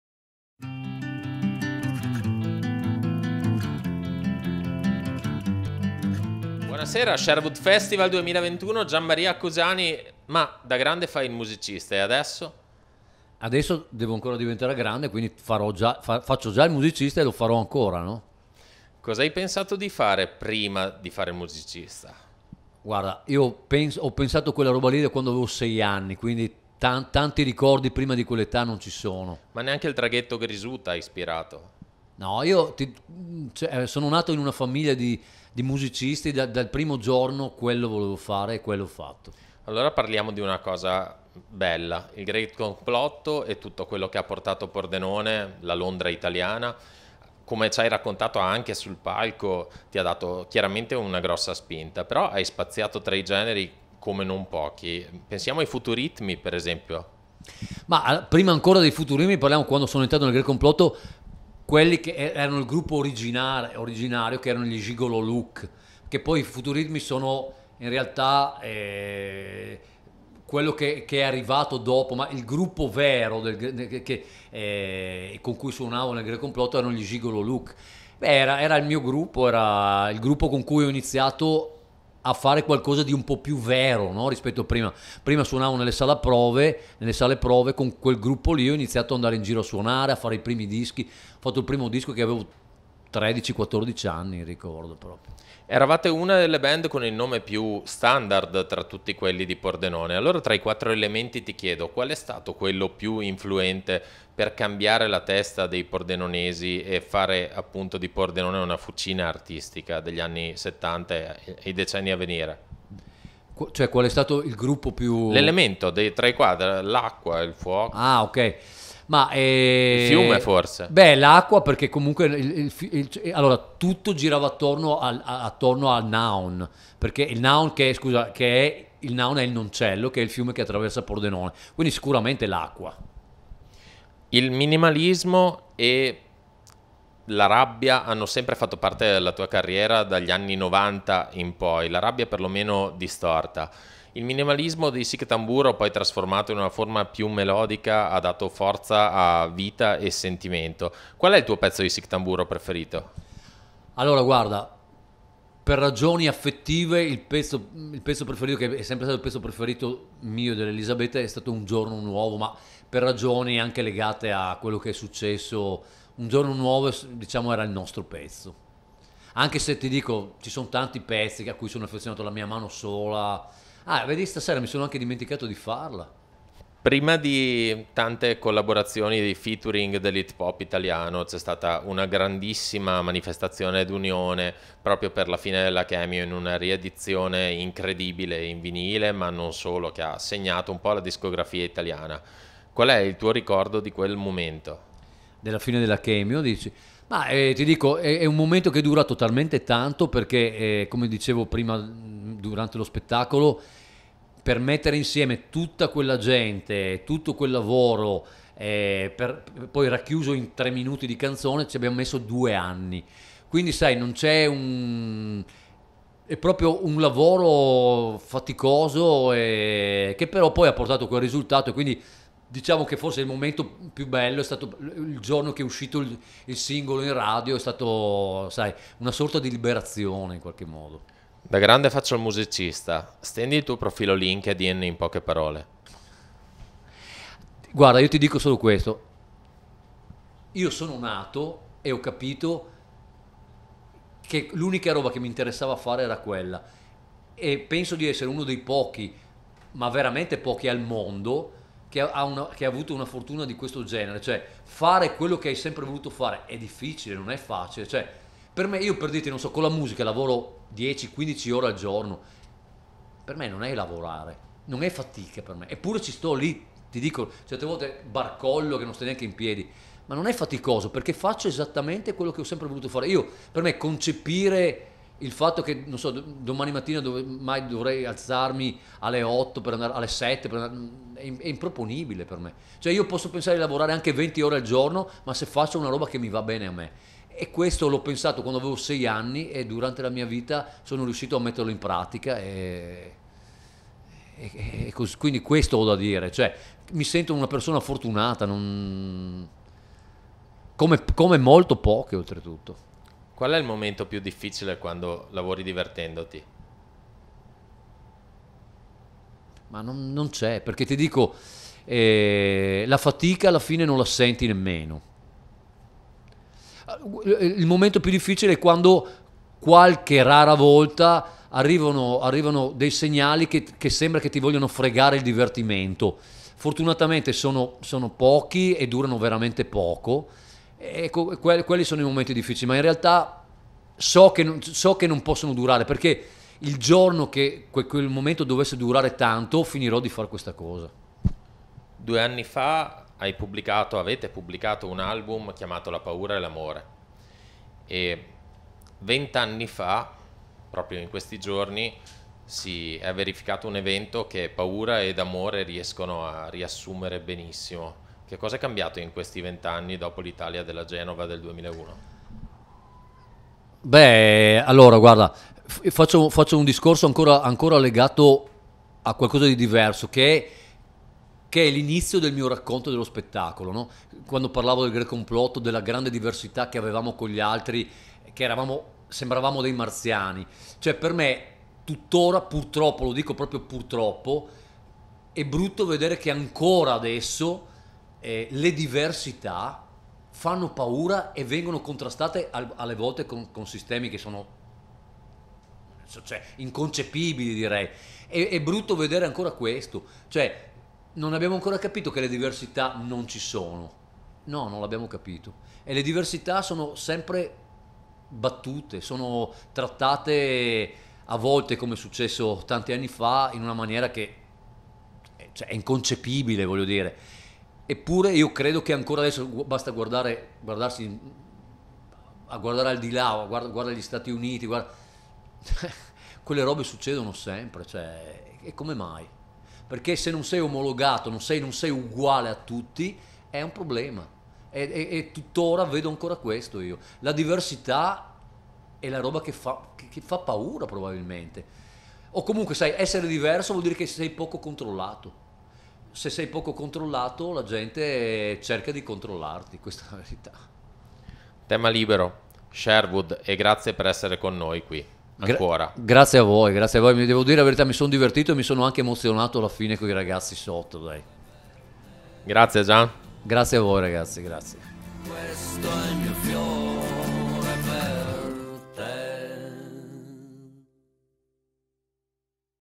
Sherwood Festival 2021 - Intervista a Gian Maria Accusani - 1/07/21
Sherwood Festival 2021 //  23 Giugno - 24 Luglio